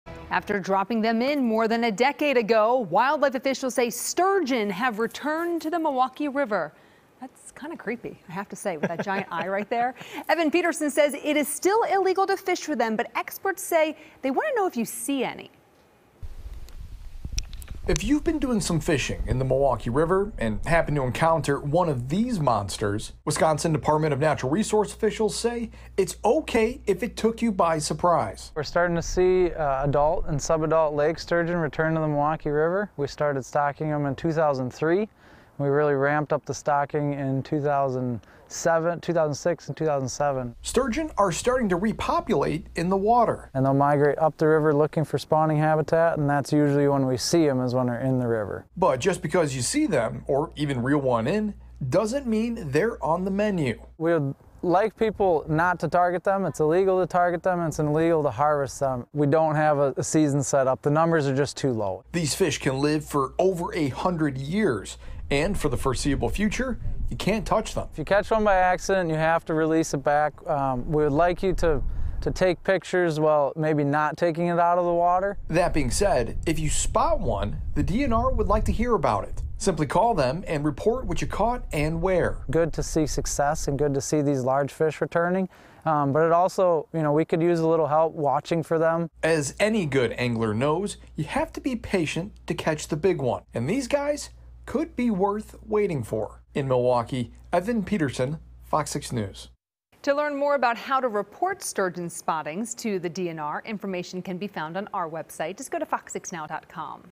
DNR: 'Illegal to target' sturgeon repopulating Milwaukee River | FOX6 News Milwaukee
After more than a decade’s worth of work and cultivation, sturgeon are slowly repopulating in Lake Michigan and connected rivers. The DNR is making an effort to protect them for years to come.